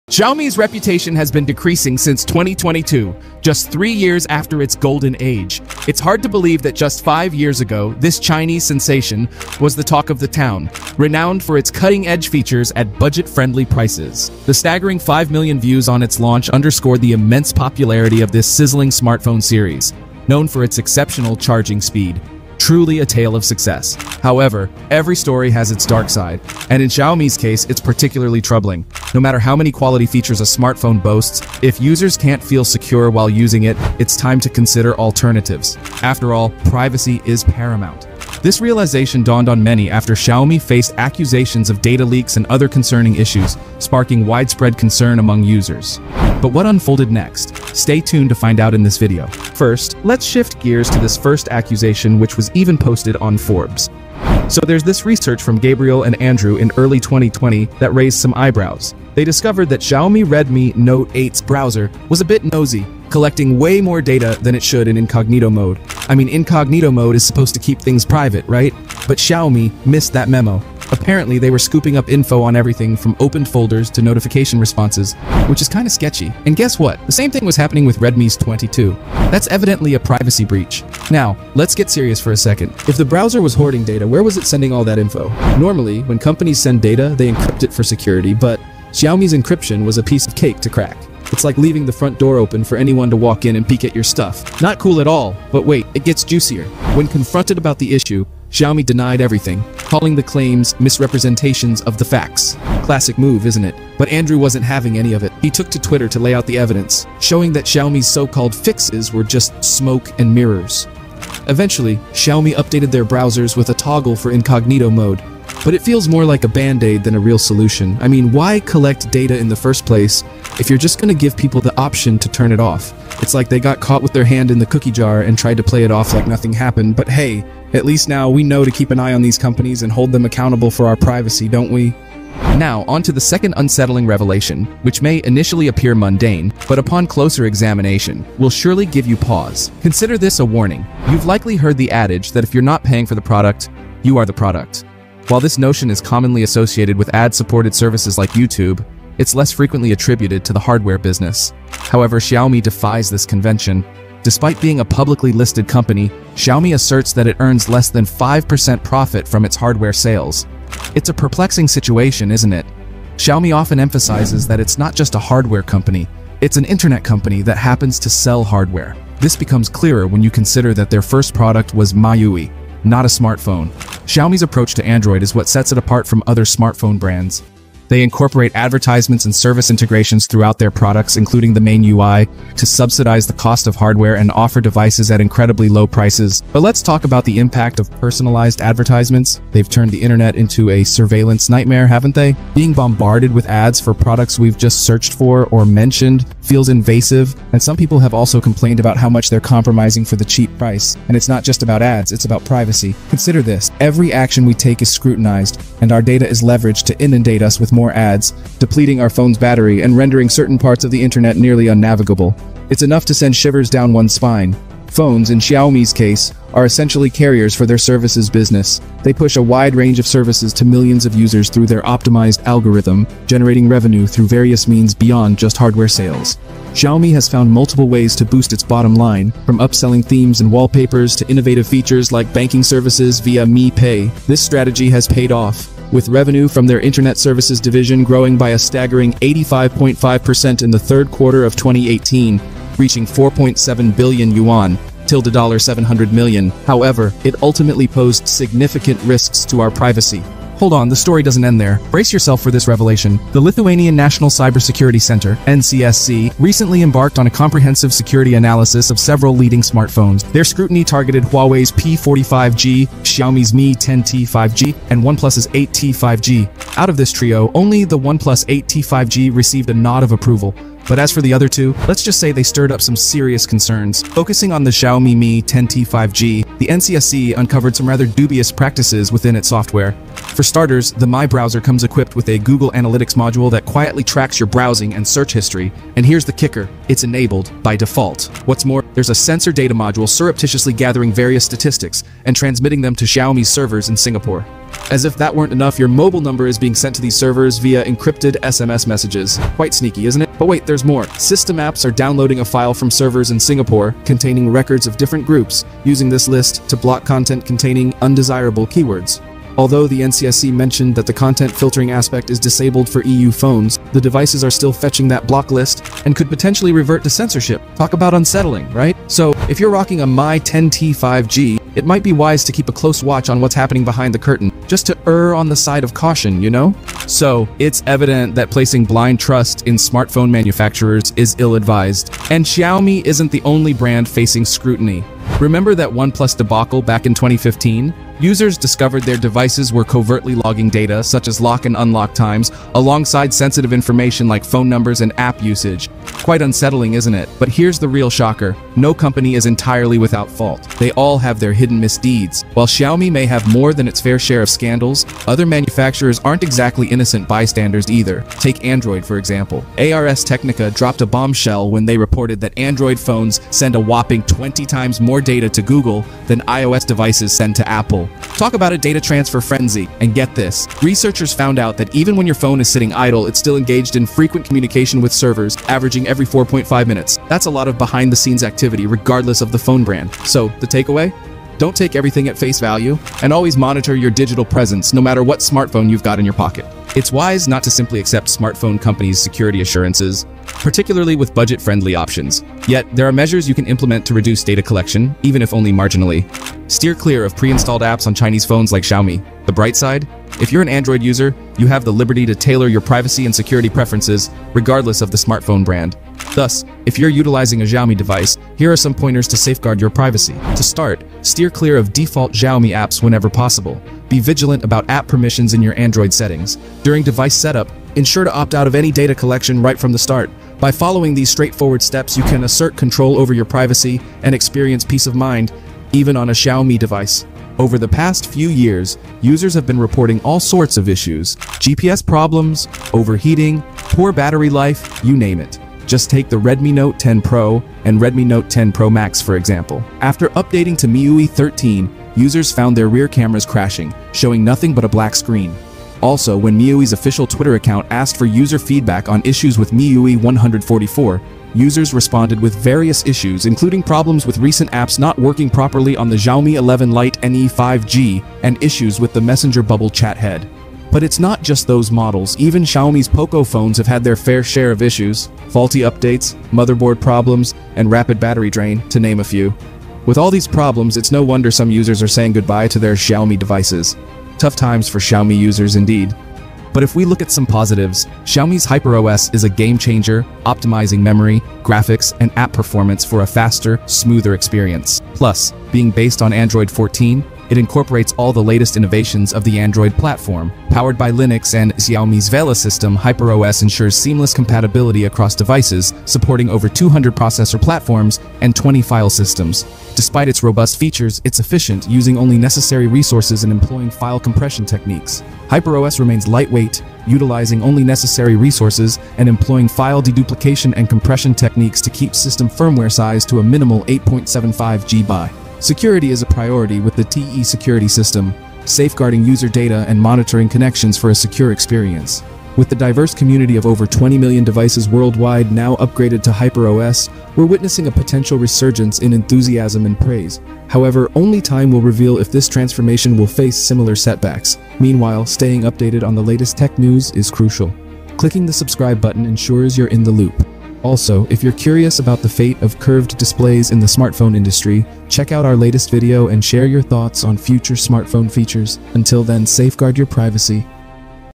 Why You Should Never Trust XIAOMI?
Xiaomi's reputation has decreased since 2022, just 3 years after its golden age. Once renowned for its budget-friendly prices and cutting-edge features, the brand faced accusations of data leaks and other issues. This sparked widespread concern among users regarding their privacy.